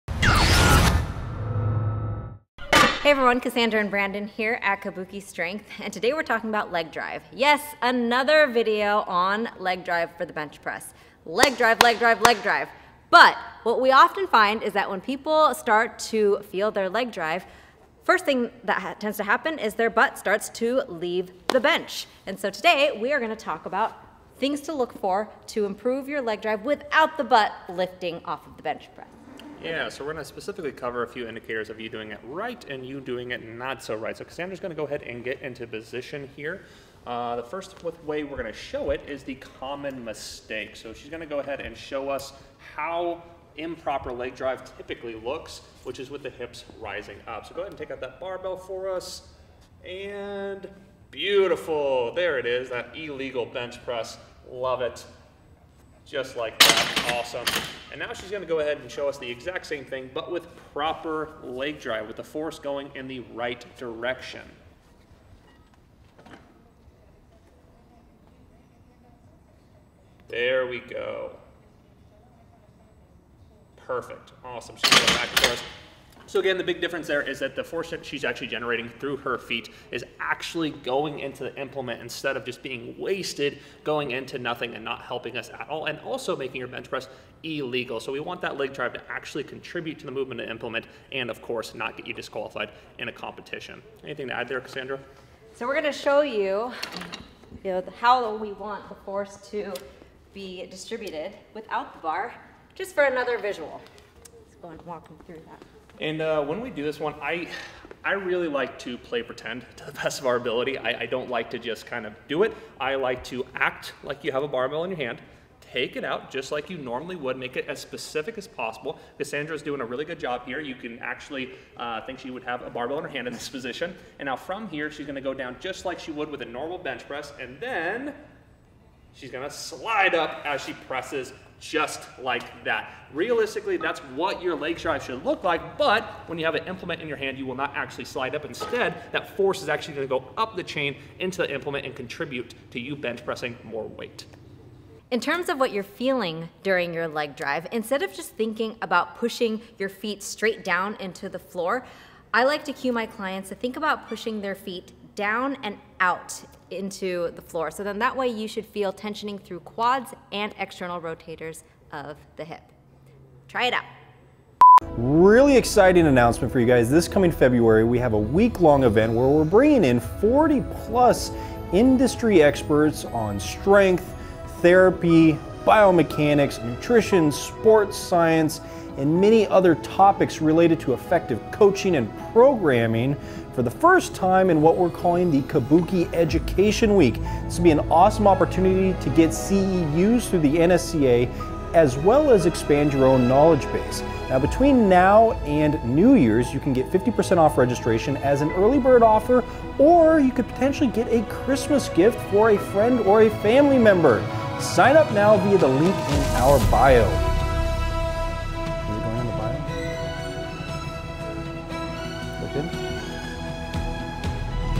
Master Leg Drive in the Bench Press for Stability and Power
Leg drive is a crucial component of our bench press aiding instability as well as helping the actual press. Often times though the leg drive is completely wasted by generating force towards the ceiling and having one's glutes leave the bench making it against competition standards. We should be focusing on three things while producing leg drive:

-Rooting feet into the floor

-Driving backwards towards the spotter

-Pressing outwards using external rotators

By focusing on these three components force production will go up the chain into the implement and more importantly you will be able to meet competition standards!